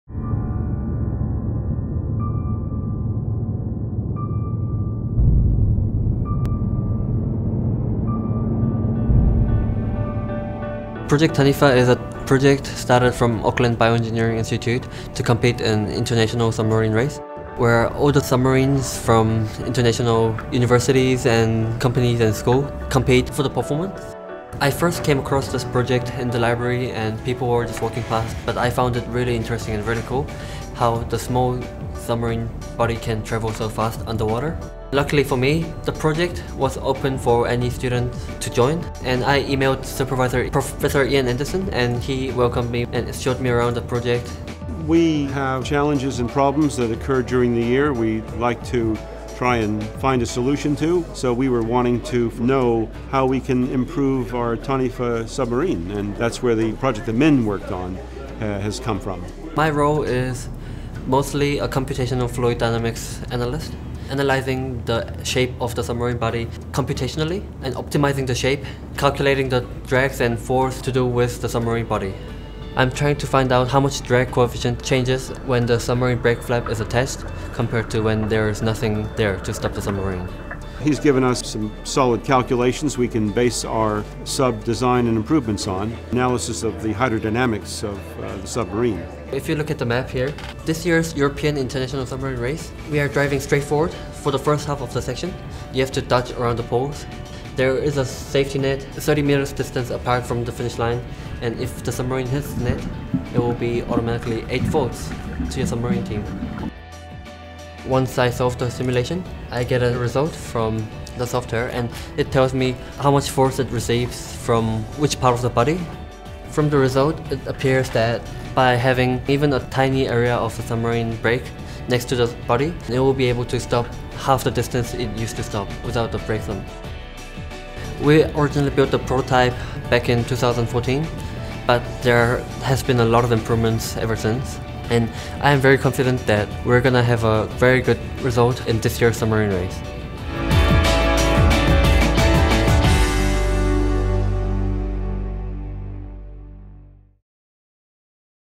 Summer Scholarships: Min's experience
Every year the University of Auckland awards $2 million in Summer Research Scholarships. Engineering student Min Sang Kim talks about working on the human-powered submarine. Project Taniwha, as a summer scholar.